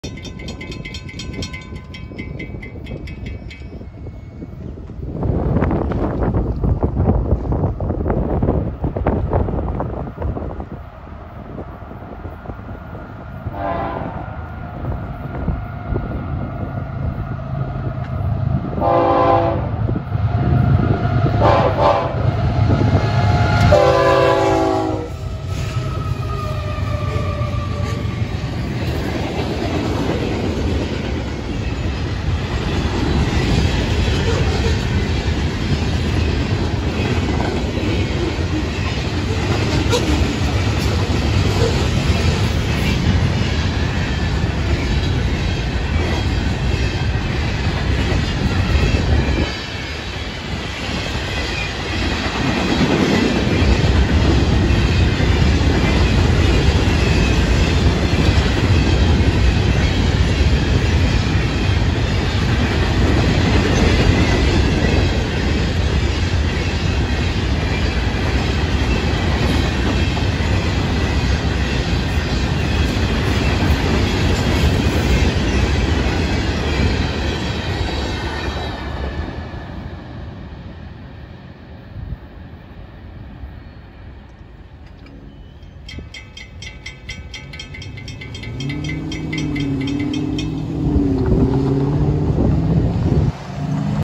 FEC TRAIN 103 @12:52pm 4-9-22
Southbound Fec engines 809, lng tender 305,&803 leading freight cars,& rock hoppers